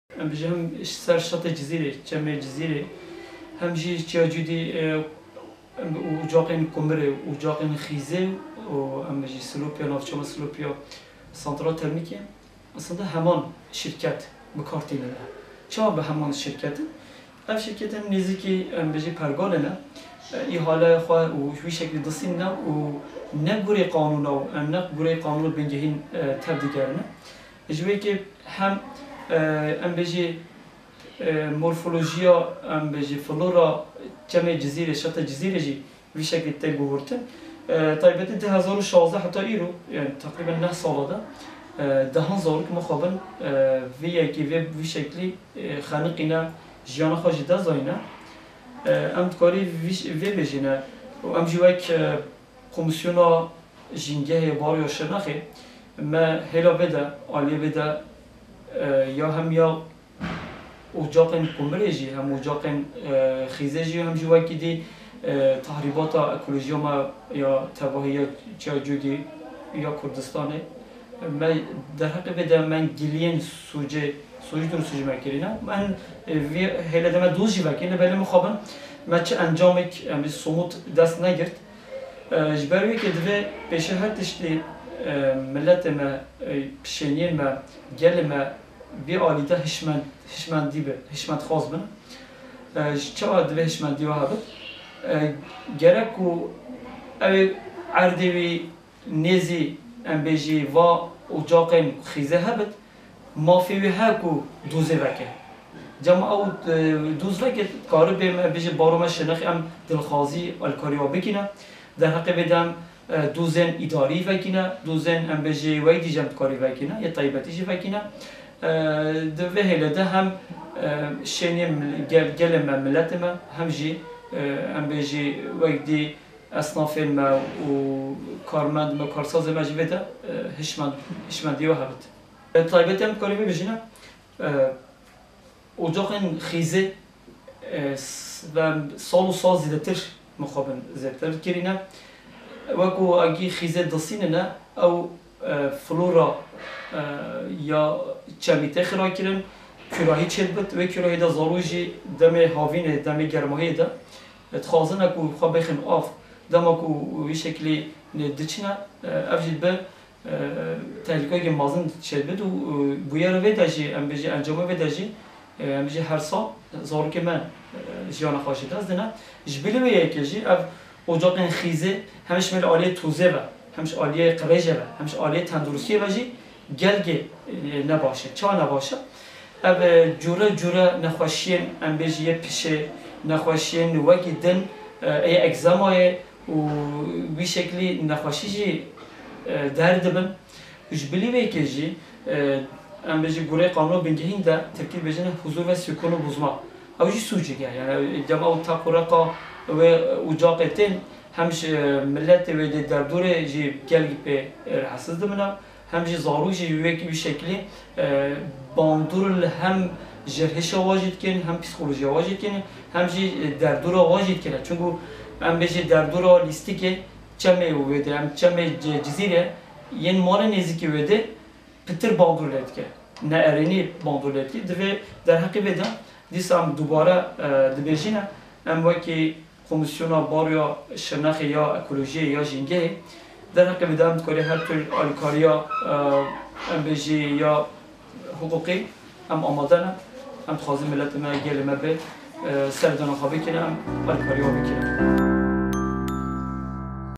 KANÊN XIZÊ ÇEMÊ DÎCLEYÊ TALAN Û WÊRAN DİKİN
Kanên xîzê yên li Cizîrê ji aliyê şîrketên nêzî desthilatdariyê ve li ser Çemê Dîcleyê hatine avakirin, derdorê talan dikin û herikîna çem diguherînin û çem teng dikin.